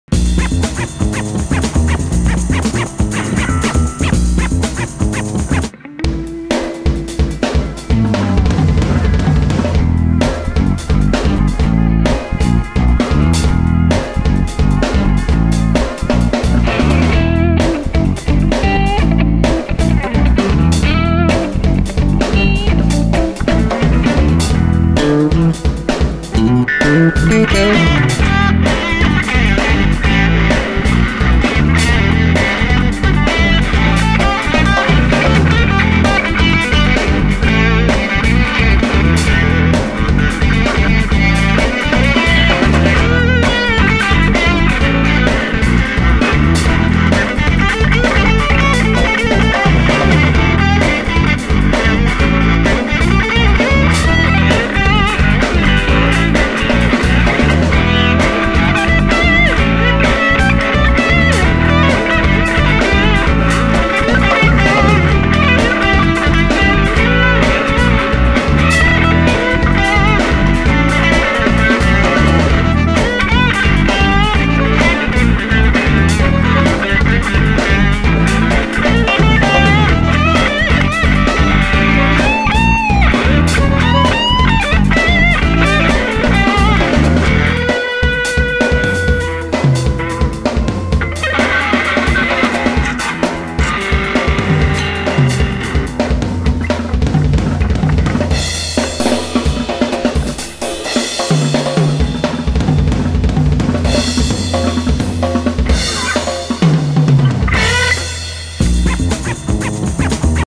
Ripping a tune for peace - Guitar example
We invite friends to rip a tune for peace. We have given you a few examples here:
Would love to hear ya

Peace Out!